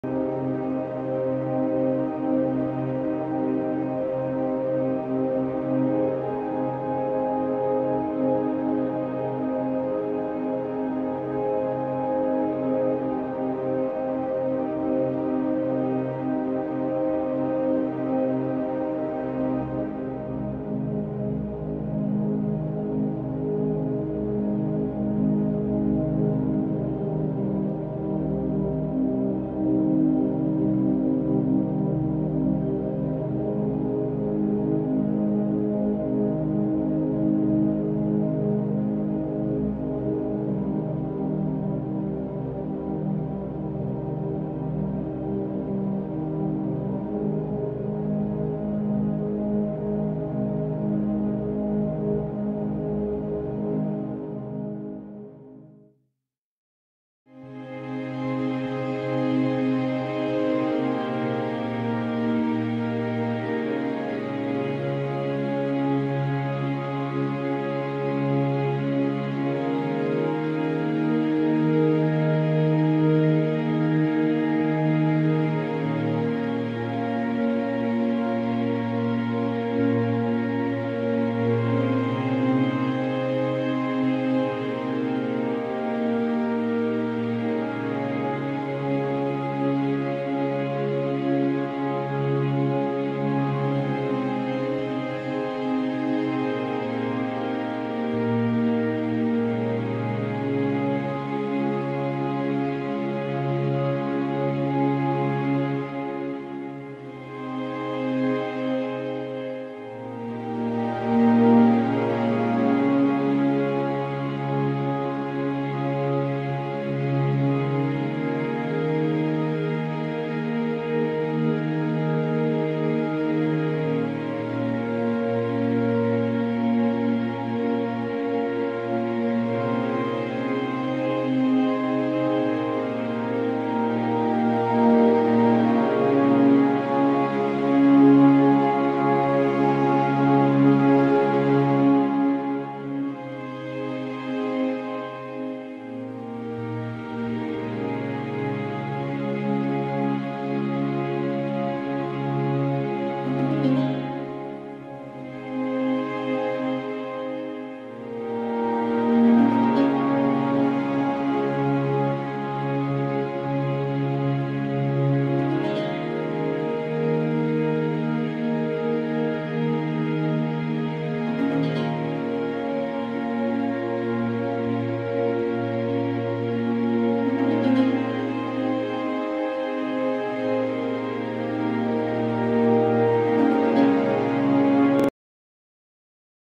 Chillout in the Botanical Gardens: Relaxing Music and Scenic Views
Enjoy the peaceful sounds of nature while taking a stroll through botanical gardens and enjoying the scenic views.